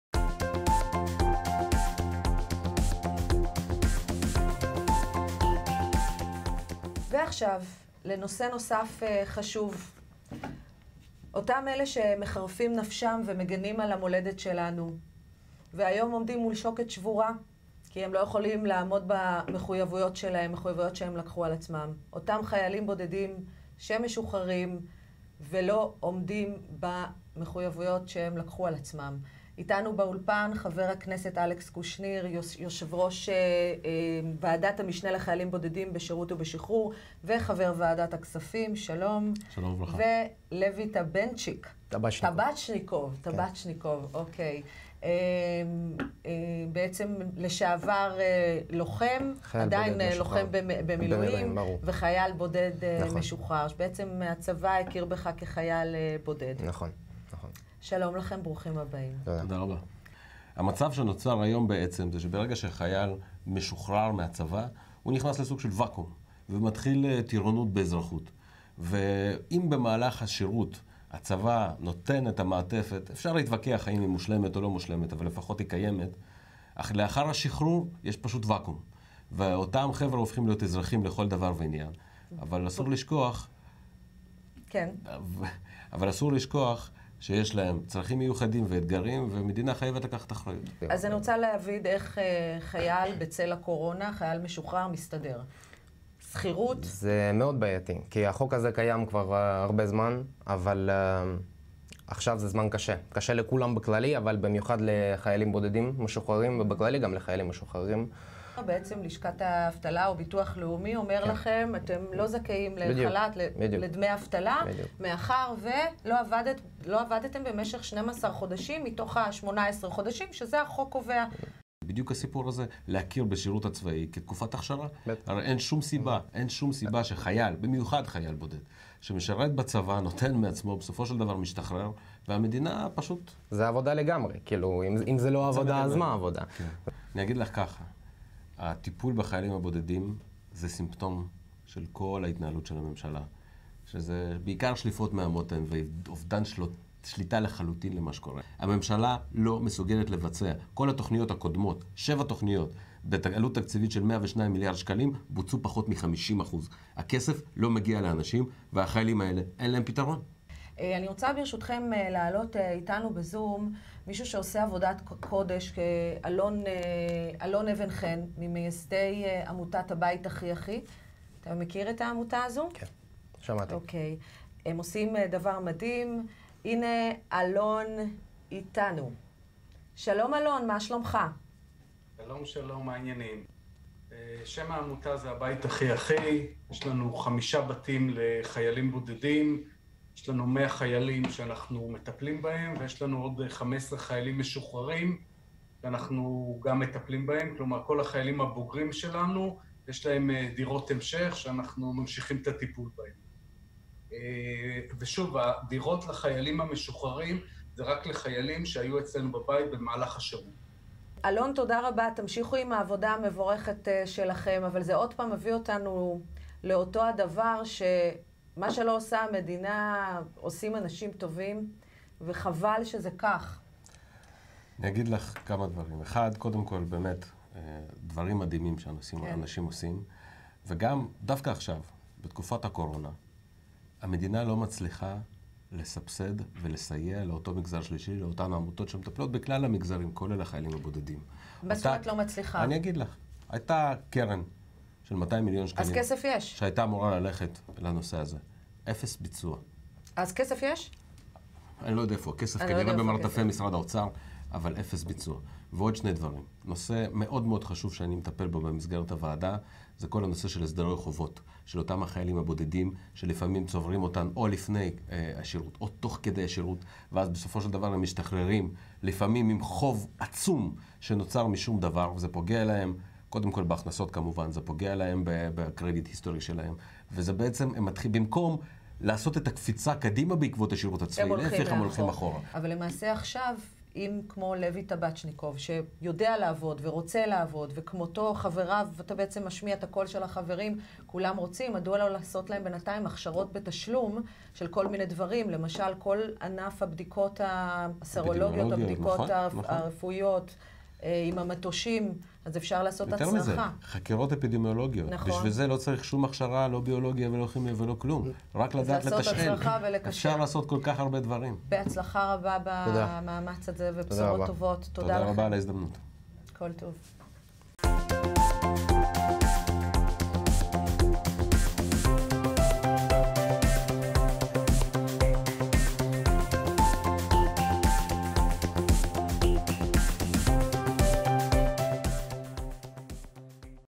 ערוץ STV - אלכס קושניר. מצוקת החיילים הבודדים המשוחררים
שאלתם היכן הכסף? אולי במרתפי האוצר. מצוקת החיילים המשוחררים הבודדים והפתרונות. הריאיון המלא